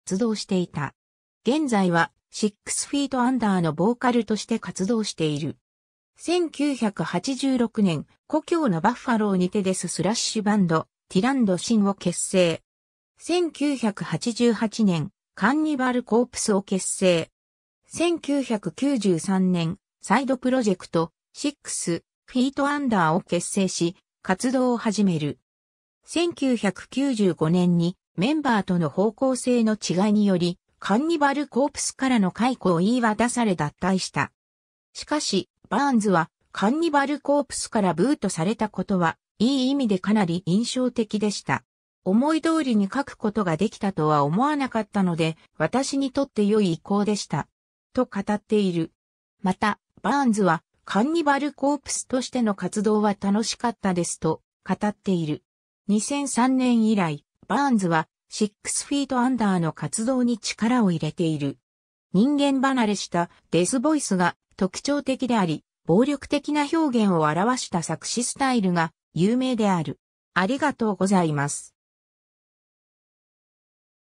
クリス・バーンズ
クリス・バーンズ（Chris Barnes、1967年12月29日 - ）は、アメリカ合衆国のミュージシャンである。
カンニバル・コープスの初期ボーカルとして活動していた。
現在はシックス・フィート・アンダーのボーカルとして活動している。
 1986年、故郷のバッファローにてデス・スラッシュバンド「Tirant Sin」を結成。
 1988年、カンニバル・コープスを結成。
 1993年、サイドプロジェクト「シックス・フィート・アンダー」を結成し、活動を始める。
1995年にメンバーとの方向性の違いによりカンニバル・コープスからの解雇を言い渡され脱退した。
しかし、バーンズは「カンニバル・コープスからブートされたことはいい意味でかなり印象的でした。思い通りに書くことができたとは思わなかったので、私にとって良い移行でした。」と語っている。
また、バーンズは「カンニバル・コープスとしての活動は楽しかったです」と語っている。
 2003年以来、バーンズは、シックス・フィート・アンダーの活動に力を入れている。
 人間離れしたデスボイスが特徴的であり、暴力的な表現を表した作詞スタイルが有名である。